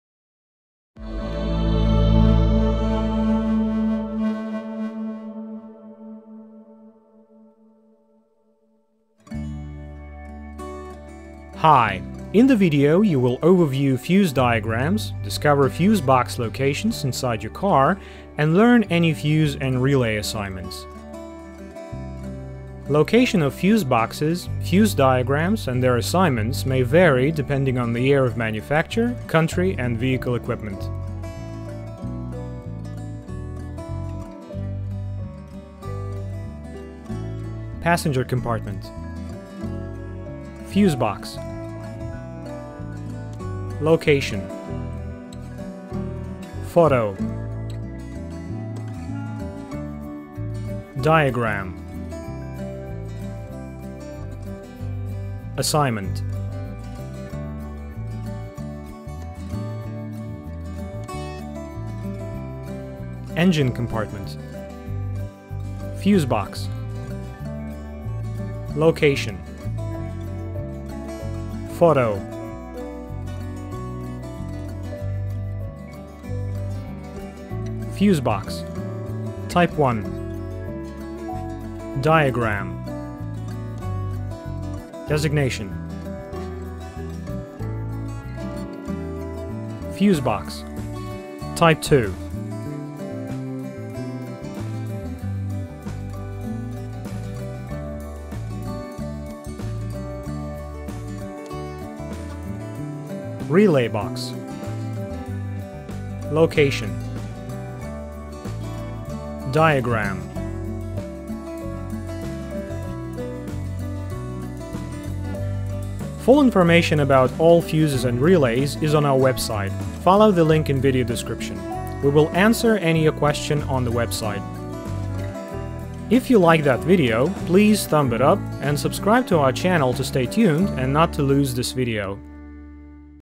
Fuse box diagram Nissan Wingroad and relay with assignment and location
Updated information on fuse boxes and relays Nissan Wingroad with photos, diagrams and a description of their decoding. The fuse is for the cigarette lighter.